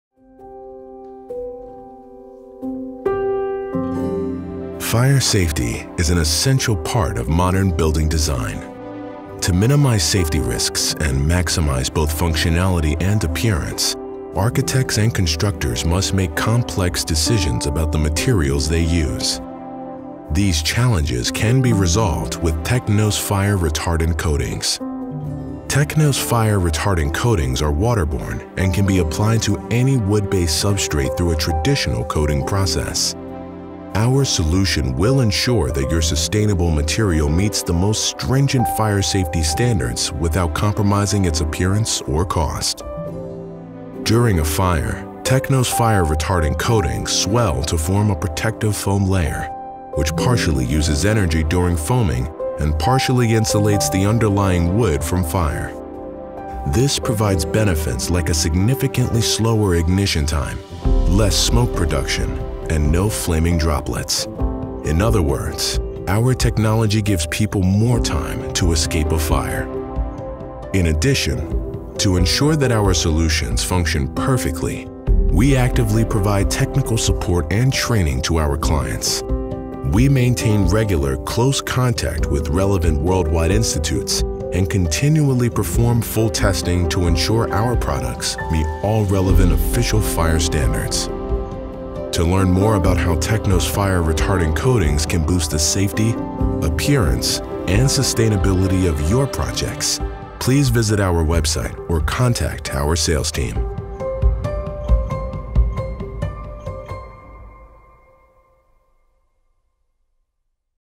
Teknos Fire-Retardant Coatings
Fire safety is an essential part of modern building design. To minimize safety risks and maximize both functionality and appearance, architects and constructors must make complex decisions about the materials they use. These challenges can be resolved with Teknos fire retardant coatings.

Teknos fire retardant coatings are waterborne and can be applied to any wood-based substrate through a traditional coating process. Our solution will ensure that your sustainable material meets the most stringent fire safety standards without compromising its appearance or cost.

During a fire, Teknos fire retardant coatings swell to form a protective foam layer, which partially uses energy during foaming and partially insulates the underlying wood from fire. This provides benefits like a significantly slower ignition time, less smoke production, and no flaming droplets. In other words, our technology gives people more time to escape a fire.

In addition, to ensure that our solutions function perfectly, we actively provide technical support and training to our clients. We maintain regular, close contact with relevant worldwide institutes and continually perform full testing to ensure our products meet all relevant official fire standards.